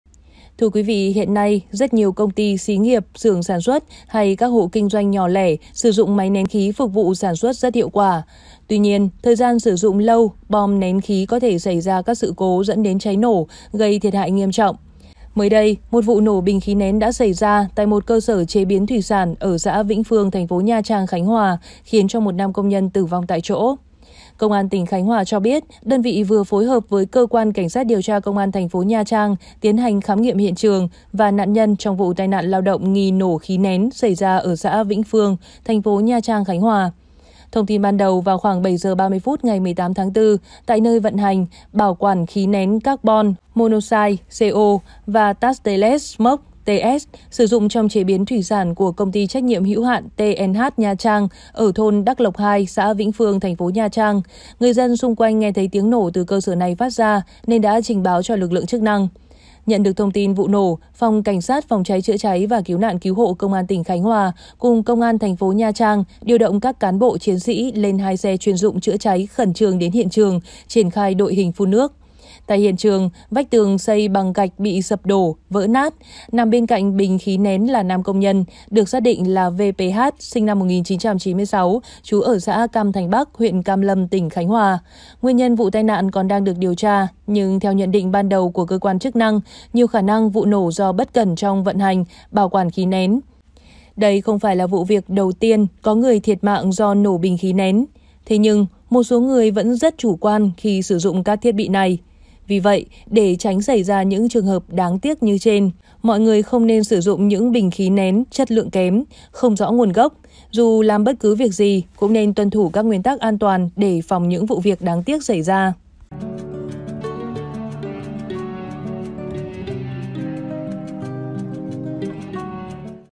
Nổ Bình Khí Nén Co2 Ở Nhà Máy Chế Biến Thủy Sản, Một Công Nhân Tử Vong Tử Vong Tại Chỗ | SKĐS
SKĐS | Công an tỉnh Khánh Hòa cho biết, đơn vị vừa phối hợp với Cơ quan CSĐT Công an TP Nha Trang tiến hành khám nghiệm hiện trường và nạn nhân trong vụ tai nạn lao động nghi nổ khí nén xảy ra ở xã Vĩnh Phương, TP Nha Trang (Khánh Hòa).

Mời quý vị và các bạn xem thêm: Phát Hiện Điều “Bí Ẩn” Bên Trong Túi Áo Của Kẻ Sát Hại Người Phụ Nữ Ở Hầm Chung Cư